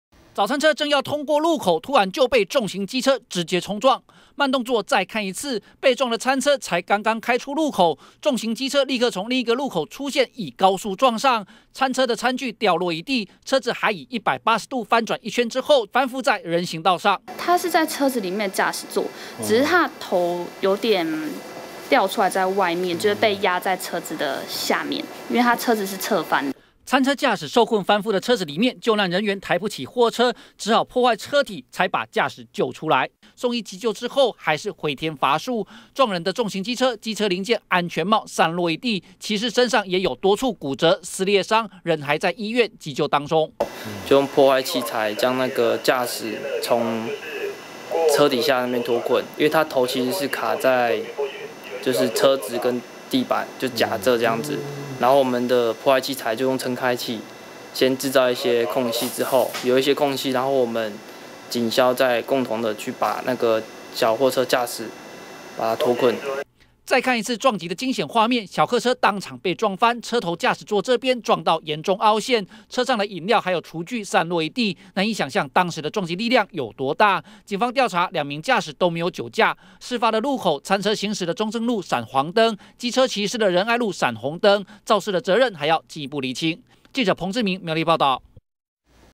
20150214中天新聞 早餐小貨車遭重機撞翻 駕駛送醫不治
苗栗縣頭份市區昨清晨傳死亡車禍，一輛大型重型機車在路口高速撞上賣早餐的小貨車，強大撞擊力道，竟將小貨車當場撞翻，駕駛頭部重創死亡，至於肇事騎士傷重命危；檢警昨相驗研判，重機時速可能破百，撞擊點又是貨車車門，才會釀成嚴重死傷。